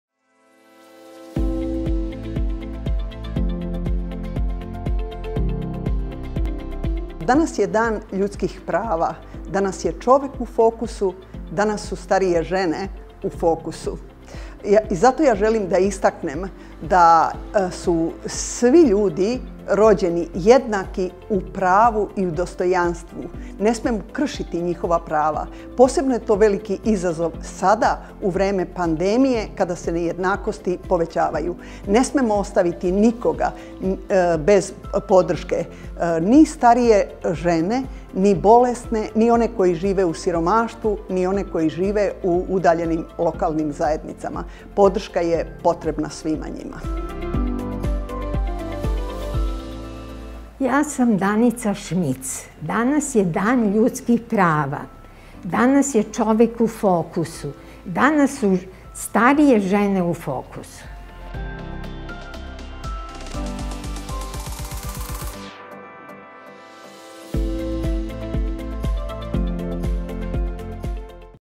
Danas obeležavamo Dan ljudskih prava - najbolja fotografija EU projekta "Čovek u fokusu"
U kategoriji „Najbolja fotografija EU projekta” izabrana je fotografija „Na stanici – volonterka na putu ka zavisnoj osobi”, projekat „Za povećano učestvovanje starijih žena u javnom i političkom životu”, Udruženje građana „Amity” (Beograd, Srbija). Na fotografiji je prikazana volonterka Danica Šmic, koja u decembru ove godine obeležava 50 godina aktivnog volonterskog angažmana na Savskom vencu, u Beogradu. Gospođa Danica ima 84 godine, a počela je da volontira u 34. godini. Naime, Danica je 1970. završila kurs za dobrovoljnu bolničarku Crvenog krsta i od tada ne prestaje da aktivno, kao volonterka, učestvuje u javnom životu.